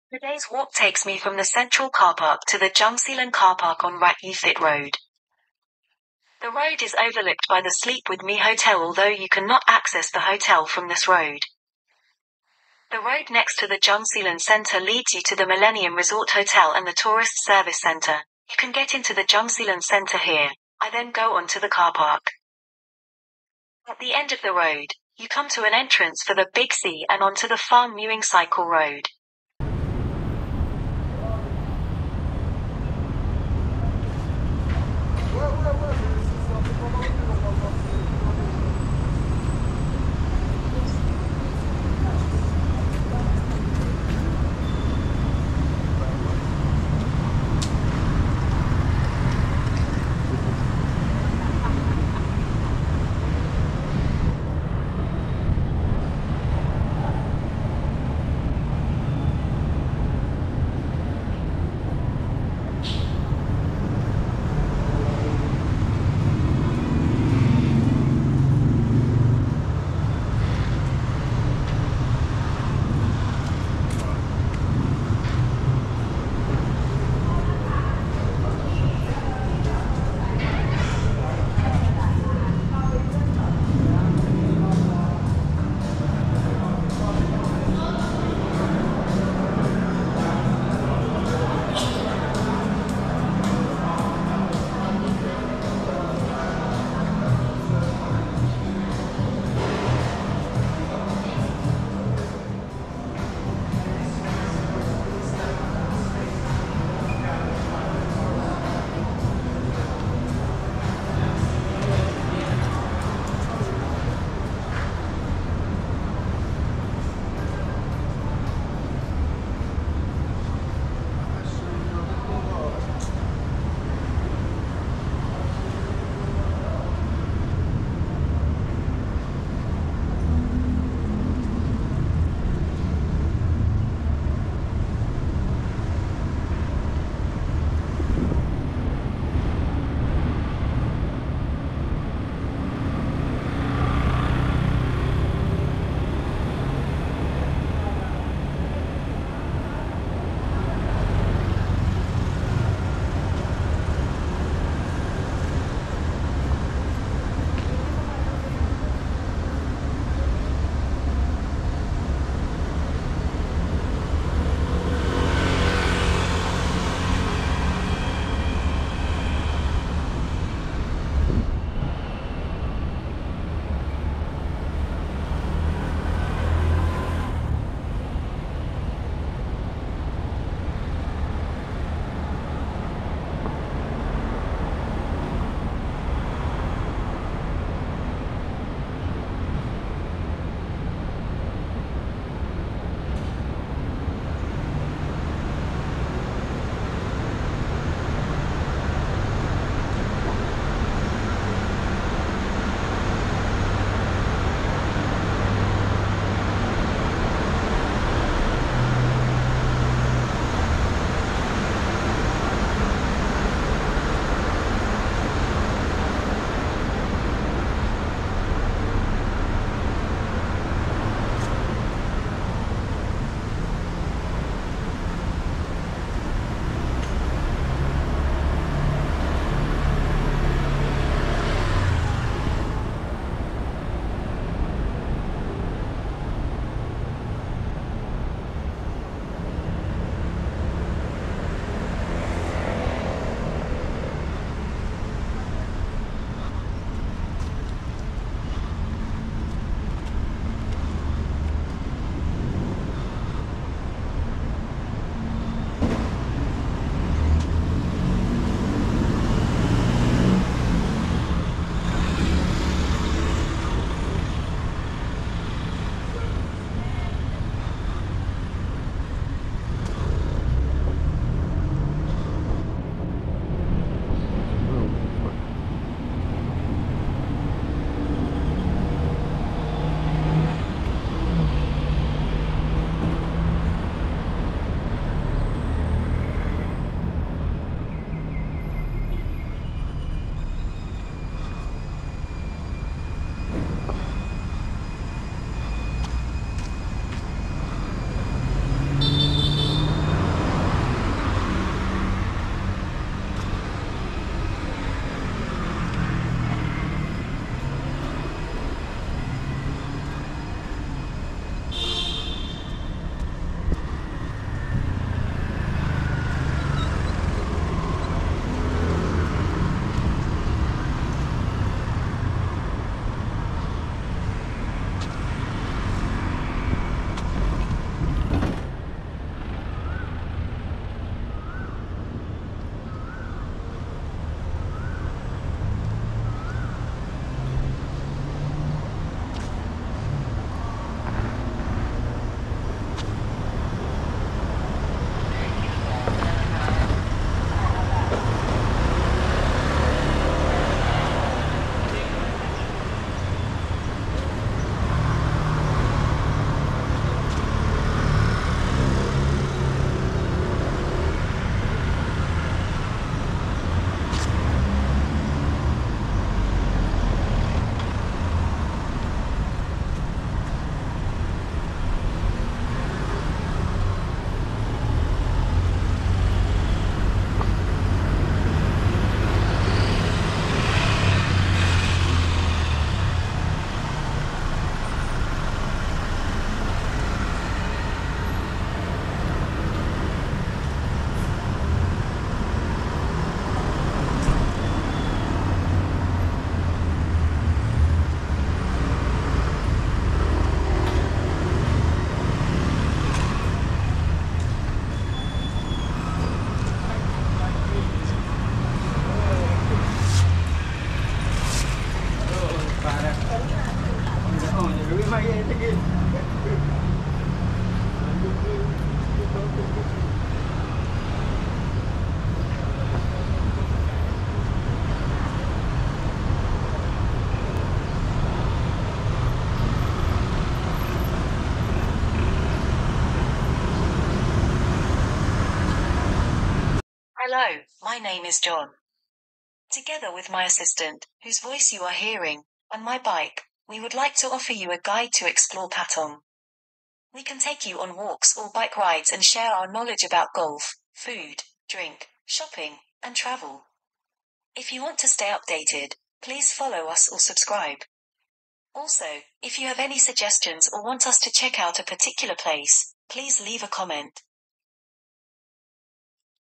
Walk from Central car park to Jungceylon car park
The two big shopping centres in Patong, Central and Jungceylon both have car parks, and the entrance to My Millennium Hotel is on the road behind the Jungceylon center